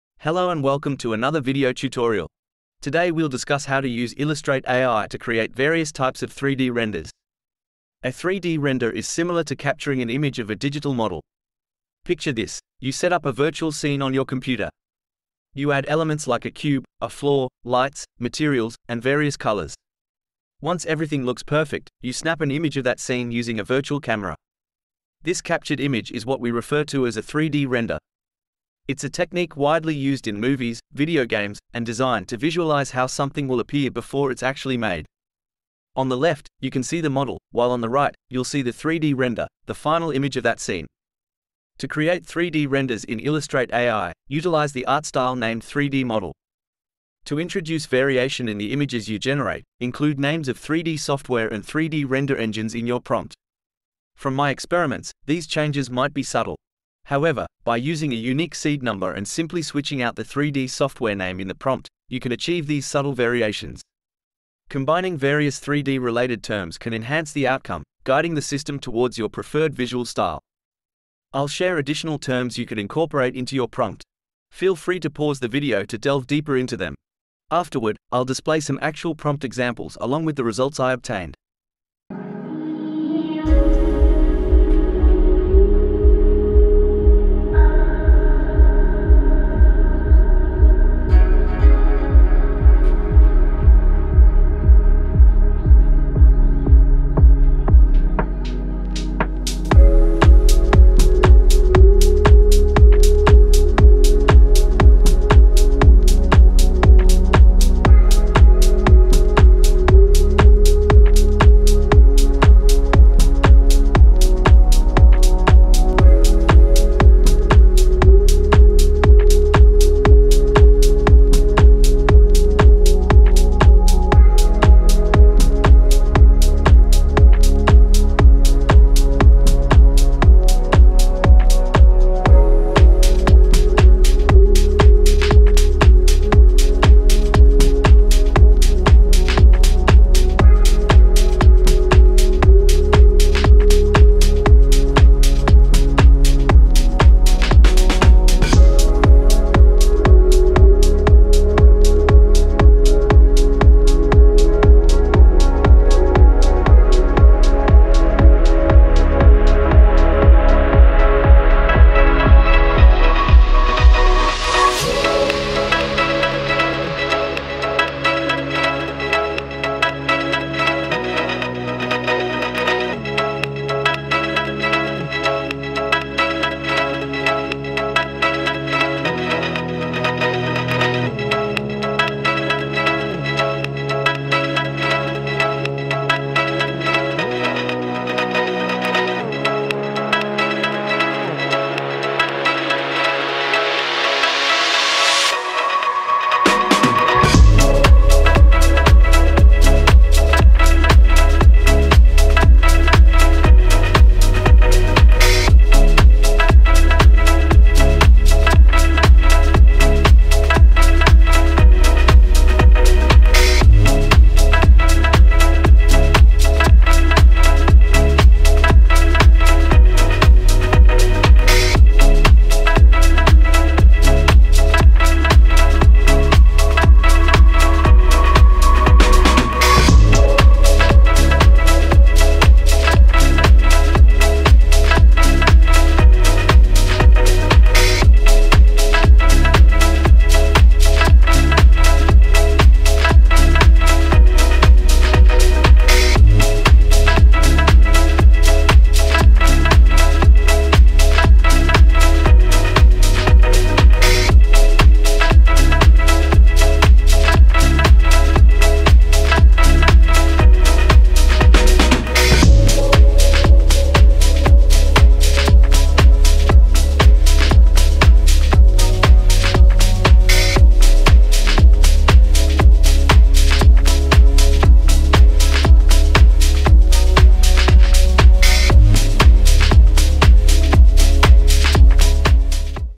3D Renders Using AI: Turning Text Prompts into Stunning Visuals!
Welcome to the newest way of making designs on the computer!
In this video, we'll explore how computers, using something called AI, can make 3D pictures from words we type. Have you ever thought about turning words into real-looking pictures?

I use a tool called "Illustrate Ai" to make amazing 3D pictures. If you like what you see, please press the 'Like' button.

Prompts also work with stable diffusion sdxl model

Chapters
00:00 Introduction to 3D Renders
00:46 3D Software and Engines List
01:10 3D Render Styles
01:21 3D Render Prompts